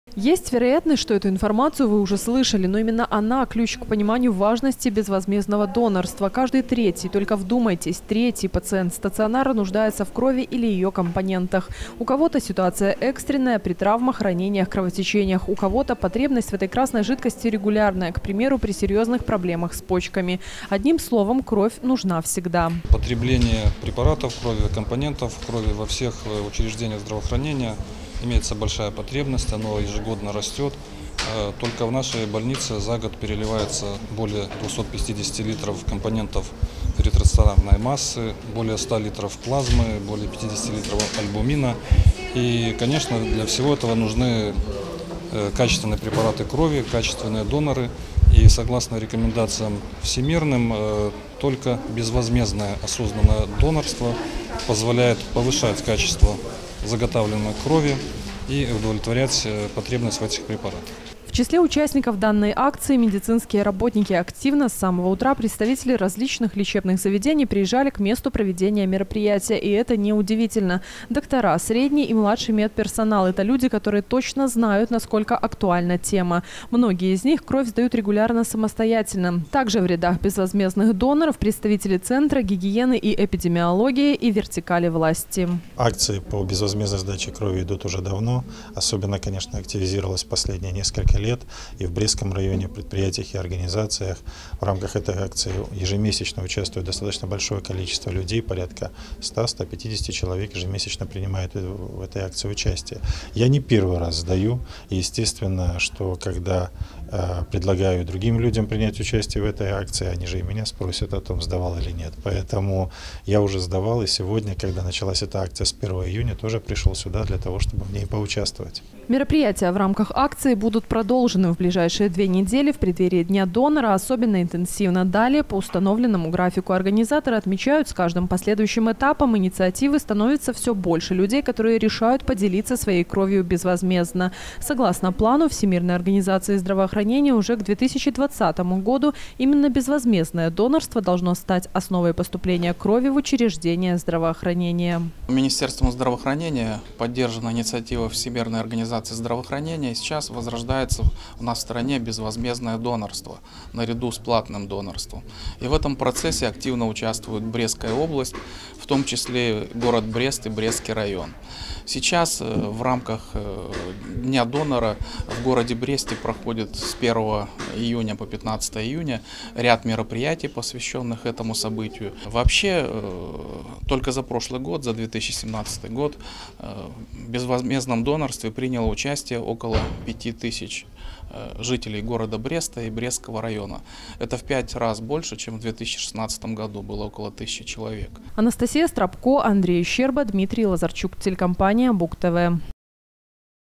2018-06-06 г. Брест. Акция «Безвозмездное донорство начинается с меня». Новости на Буг-ТВ.
Безвозмездное донорство начинается с меня. Акции под таким названием проходят на Брестчине регулярно. Участниками стали тысячи человек. Только в Бресте и районе с начала 2018-го число безвозмездных доноров крови приблизилось к 2-ум тысячам. В начале июня стартовал очередной этап. С 1-го по 15-ое число в областном центре будут организованы передвижные пункты забора крови. К примеру, недавно такой работал на базе первой городской больницы. Подробности далее.